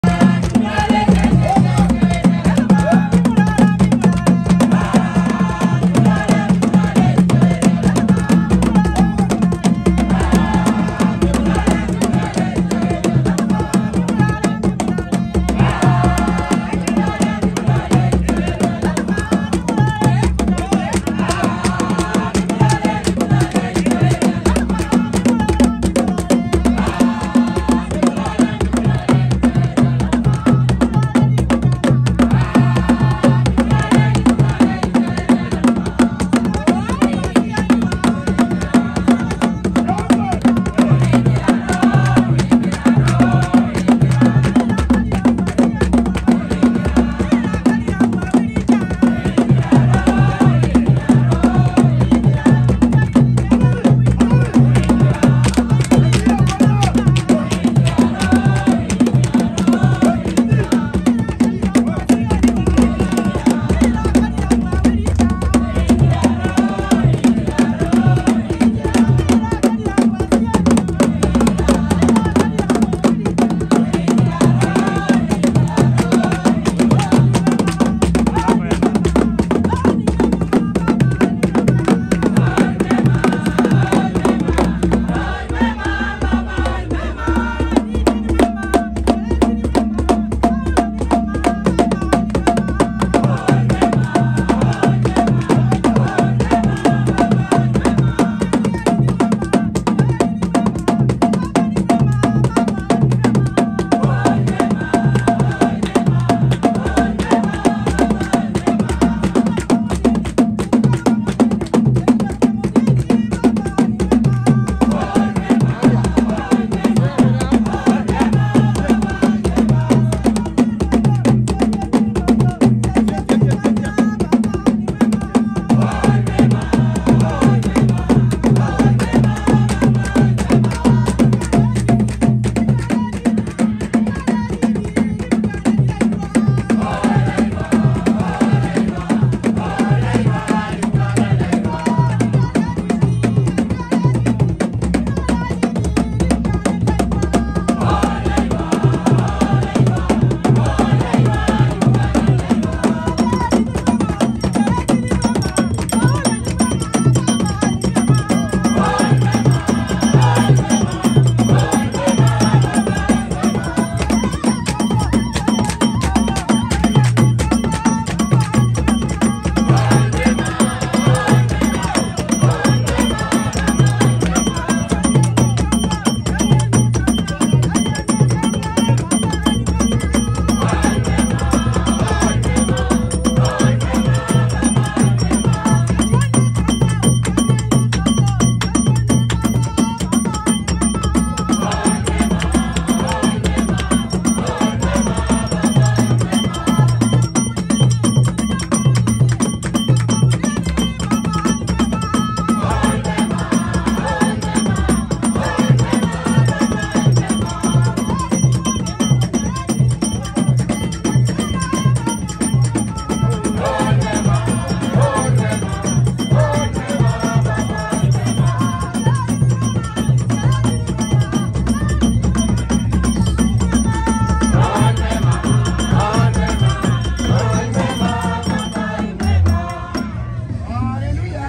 GYCC Uthiru Vumilia Sub Branch Kwenye Ubatizo Wa Watoto Nairobi County
Green Yellow  Cross ➕ Church ⛪ Iratsa HQ A Mission Shinyalu Kakamega Kenya.